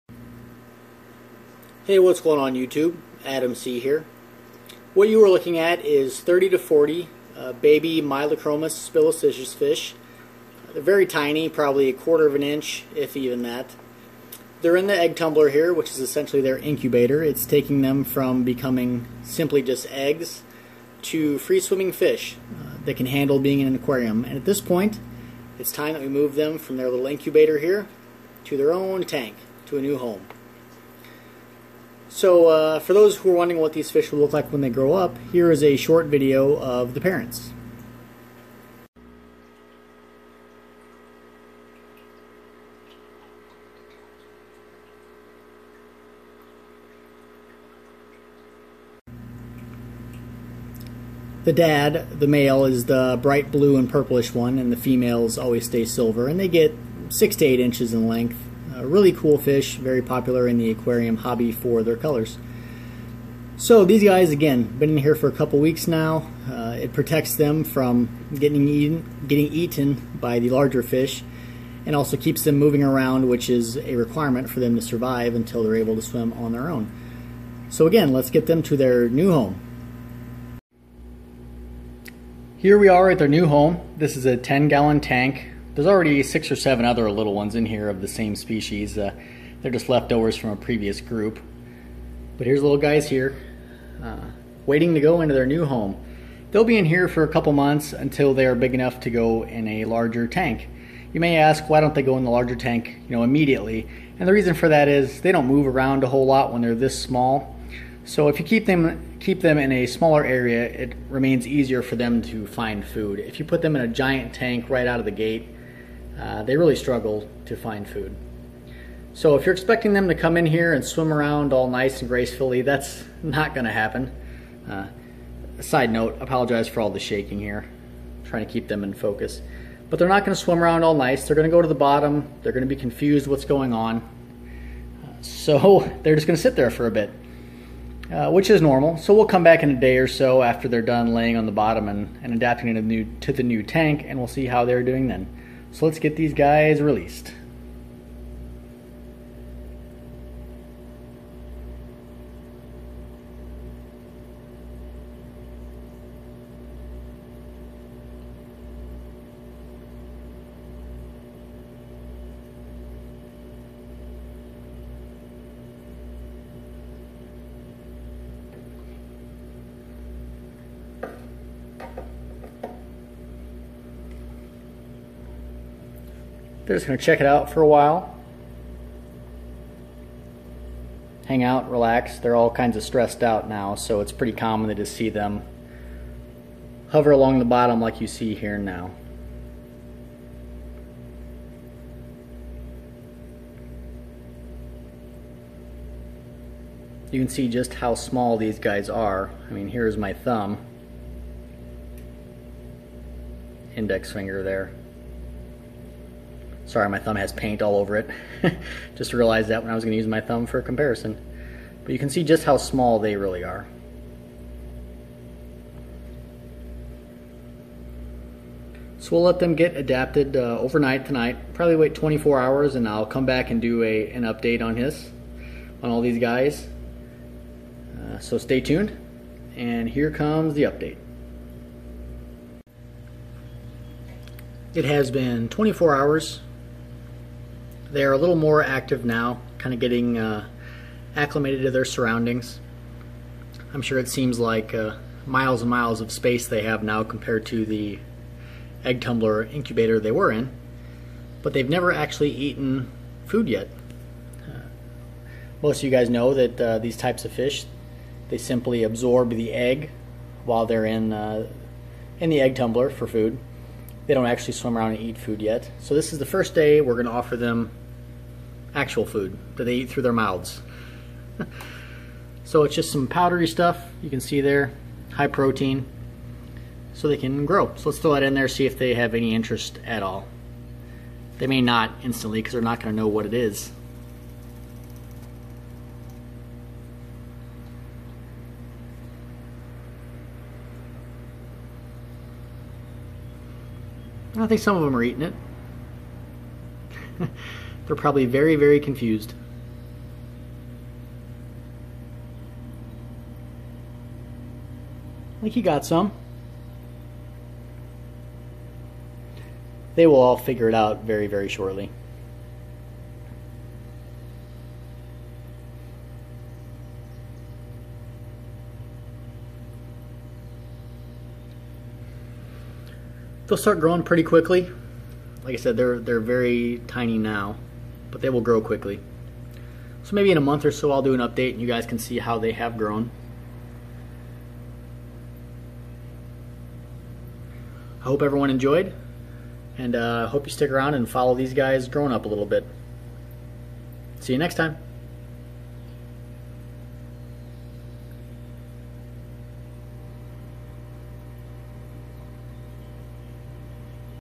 Releasing Baby Cichlid Fish Into Their New Home.....Plus Their First Meal
They Are Free Into Their New Home